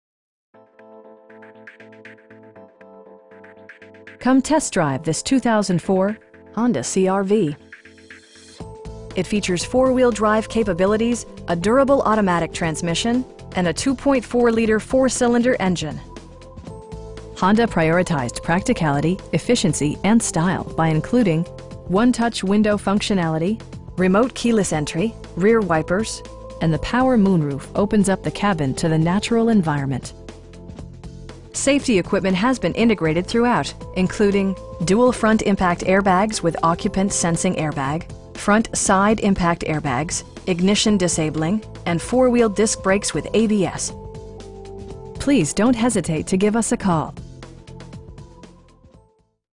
2004 Honda CR-V EX in Wilmington, DE 19806
Lexus of Wilmington
2100 Pennsylvania Ave

Come test drive this 2004 Honda CR-V. It features four-wheel drive capabilities, a durable automatic transmission, and a 2.4 liter 4 cylinder engine. Honda prioritized practicality, efficiency, and style by including: 1-touch window functionality, front bucket seats, remote keyless entry, cruise control, an overhead console, rear wipers, and the power moon roof opens up the cabin to the natural environment. Safety equipment has been integrated throughout, including: dual front impact airbags with occupant sensing airbag, front SIDE impact airbags, ignition disabling, and 4 wheel disc brakes with ABS. We know that you have high expectations, and we enjoy the challenge of meeting and exceeding them! Please don't hesitate to give us a call.